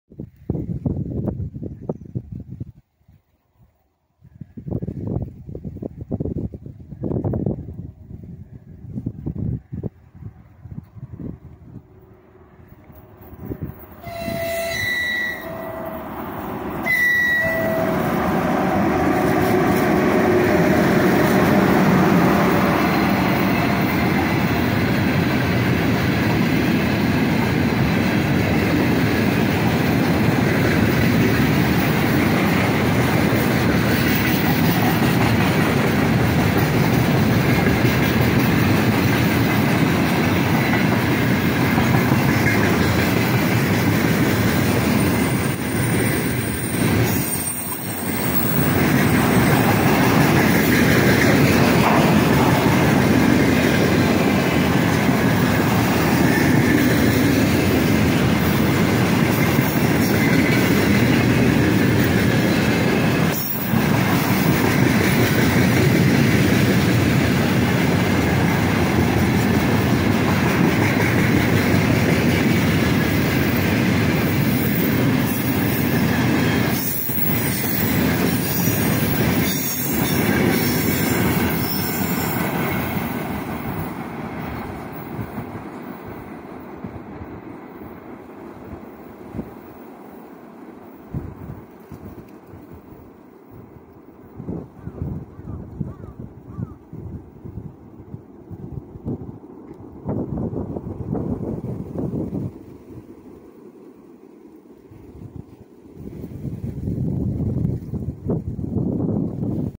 Электровоз 3ЭС5К-391 "Ермак" с наливным составом морозным февральским утром на Транссибе.
Электровоз 3ЭС5К-391 "Ермак" с порожним наливным составом следует по Транссибирской магистрали на Север солнечным морозным февральским утром.
Россия,Приморский край,перегон Барановский - Баневурово.
Дорога приписки:Забайкальская ж/д.
Депо:ТЧЭ-3 Чита.
Дата и время съёмки:25 февраля 2023 года,11:19.